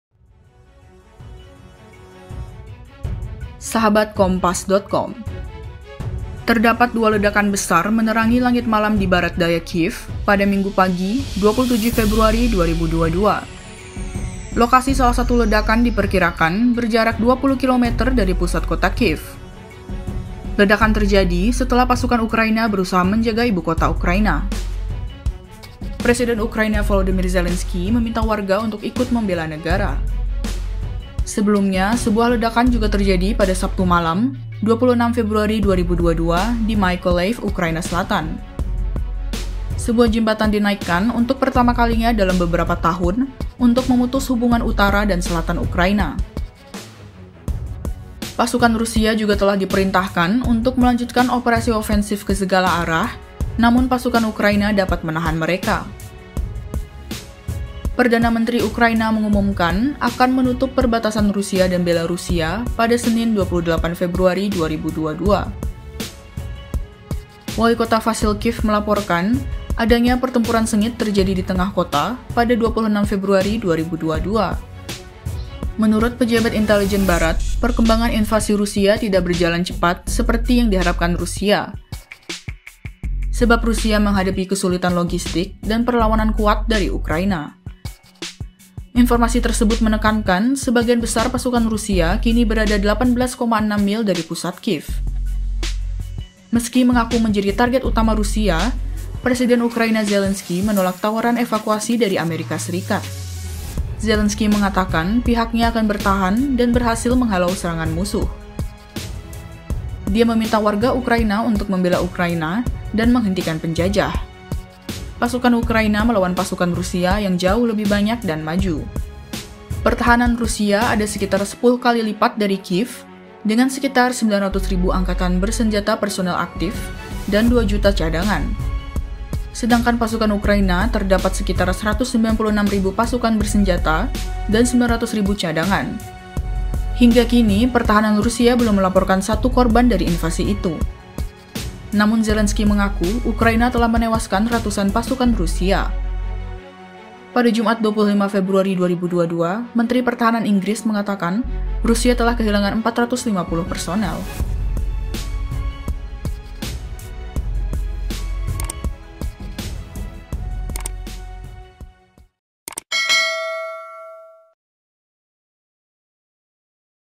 Pasukan Rusia Mendekat ke Semua Sisi, Ibu Kota Ukraina Siaga Tinggi
Pada Minggu pagi (27/2/2022), terdapat dua ledakan besar menerangi langit malam di barat daya Kiev, dimana salah satu ledakan tampaknya berjarak sekitar 20 kilometer dari pusat kota.

Ledakan terjadi setelah pasukan Ukraina berusaha menjaga ibu kota. Kini pertempuran antara Rusia dan Ukraina berlanjut di seluruh Ukraina untuk hari ketiga pada Sabtu (26/2/2022).

Sebelumnya juga terdapat sebuah ledakan pada Sabtu malam (26/2/2022) di Mykolaive, Ukraina selatan.

Penulis : Bernadette Aderi Puspaningrum
Penulis Naskah: Novinda Sekar Putri
Narator: Novinda Sekar Putri
Video Editor: Novinda Sekar Putri
Produser: Yusuf Reza Permadi